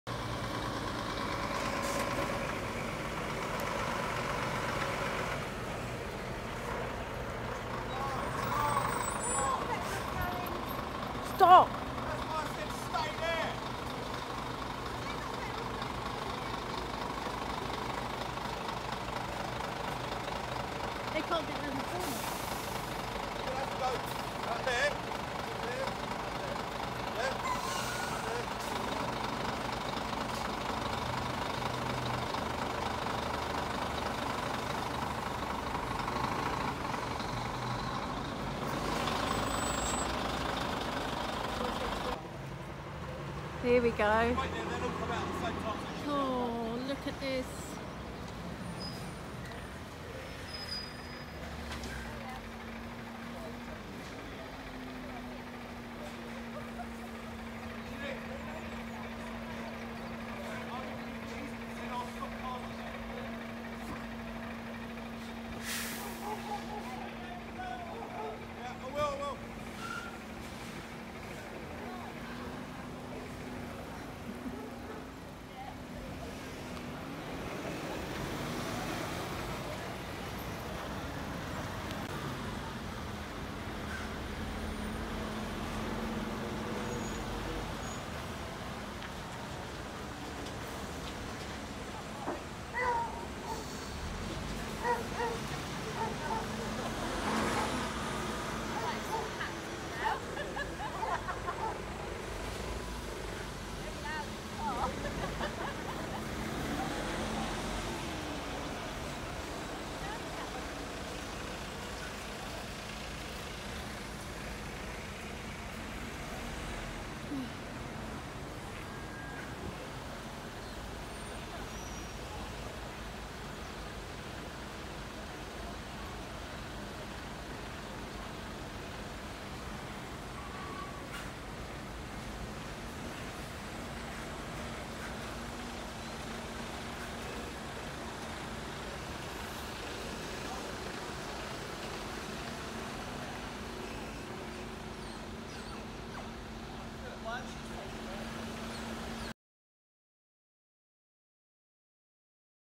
Truckers clog up village after given wrong postcode for new Brexit lorry park | SWNS
Lost truckers are clogging up a small village because the government is giving them the wrong postcode for their new Brexit lorry park. Frustrated residents living in Mersham near Ashford, Kent, have seen their narrow country lanes blocked by HGVs that have been given incorrect directions. Around 30 lorries arrived in the village since the 66-acre lorry park opened for coronavirus testing on Monday. Video footage taken by residents showed lorries meeting head-on and having to try and negotiate tight corners. One clip featured a row of three lorries stuck down a narrow road.